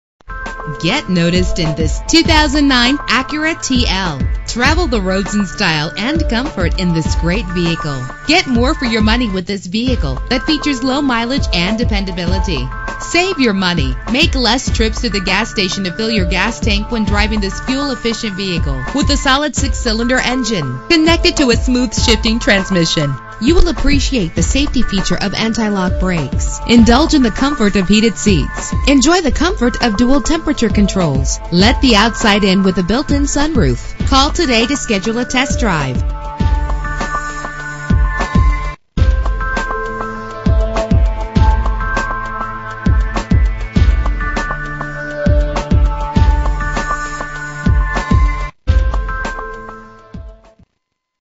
2009 Acura TL - Memphis TN
Year: 2009
Make: Acura
Model: TL
Trim: 3.5
Engine: 3.5 liter 6 cylinder
Transmission: AUTO 5SPD
Color: Silver
Mileage: 9348
Address:
7727 US Highway 64